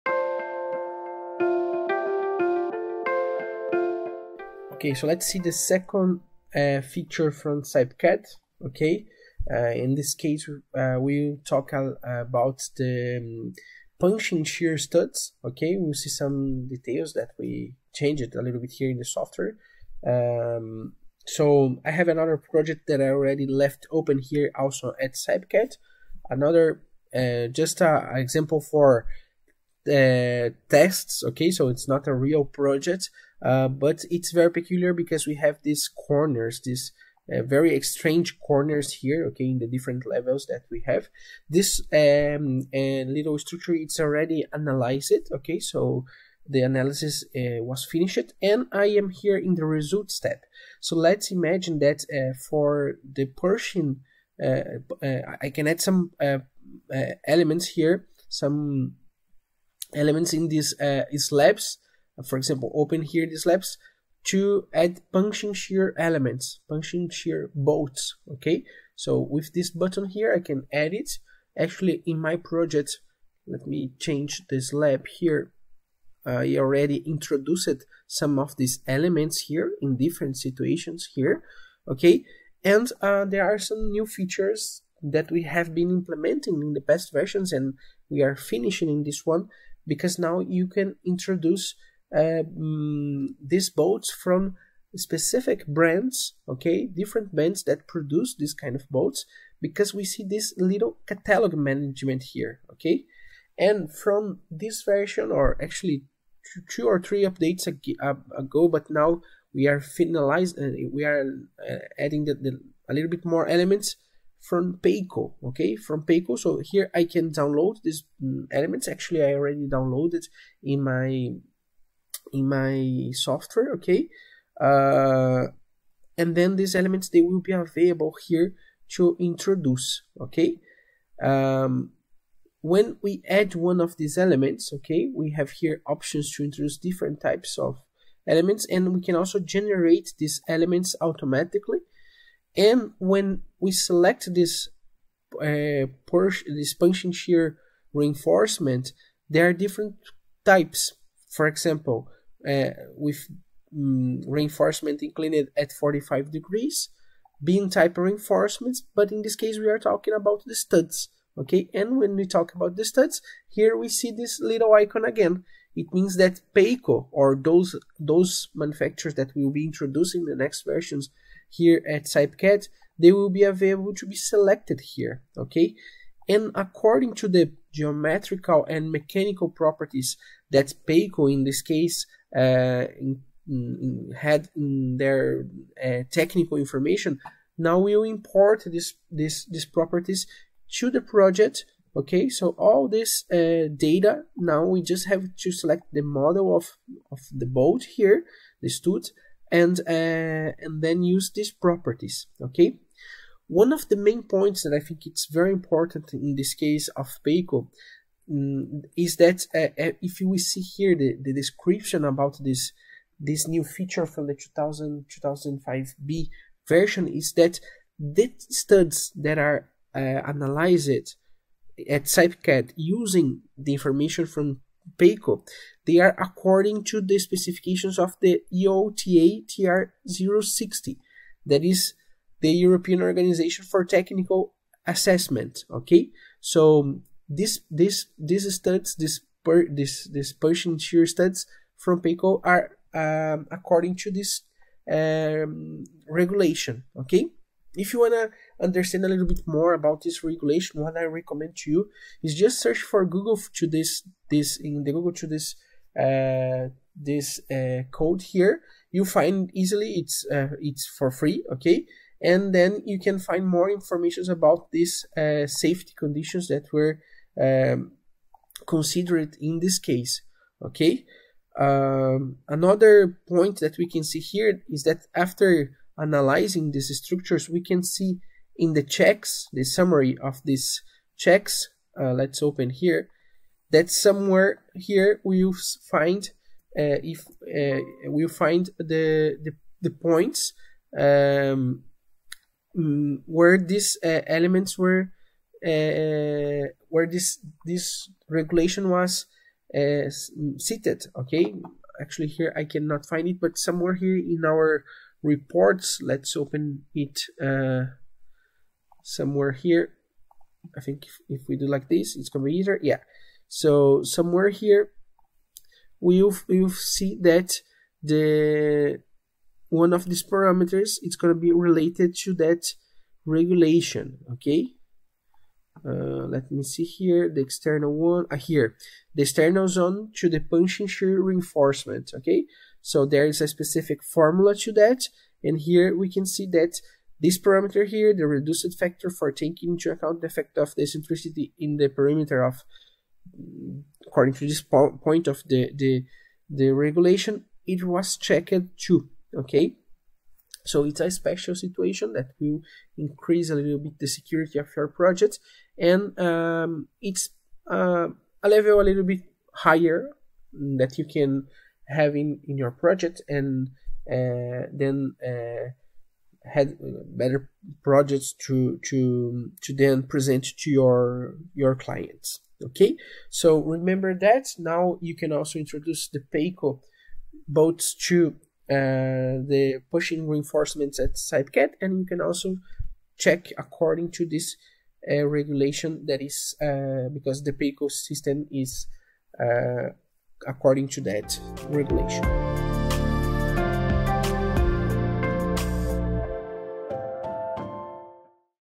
Checks for PEIKKO punching shear studs according to EOTA in CYPECAD
This content was extracted from the "Discover the main new features and improvements implemented in CYPE version 2025.b" webinar.